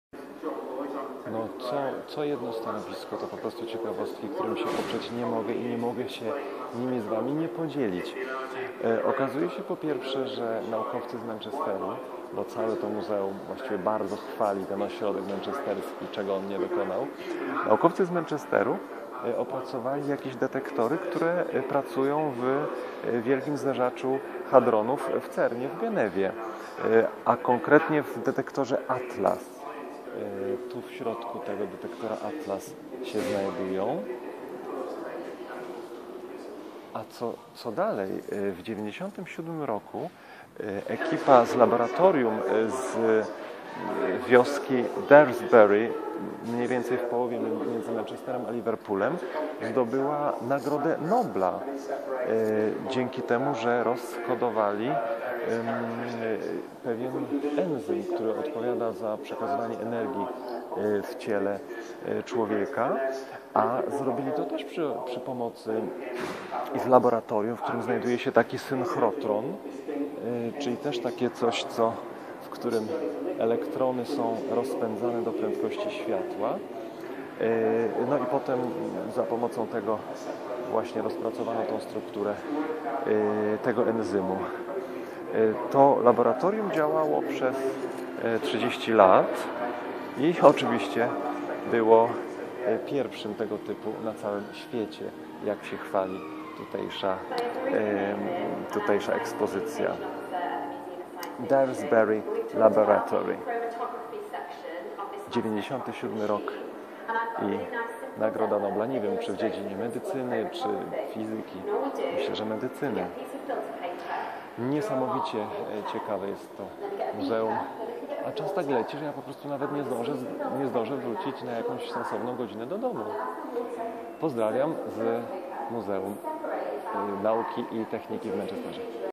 Synchrotron i nagroda Nobla
O osiągnięciach laboratorium w Daresbury i działającego tam synchrotronu. Nagroda nobla w 1997 za rozkodowanie enzymu ATP, choć wikipedia związku z Manchesterem nie potwierdza :-)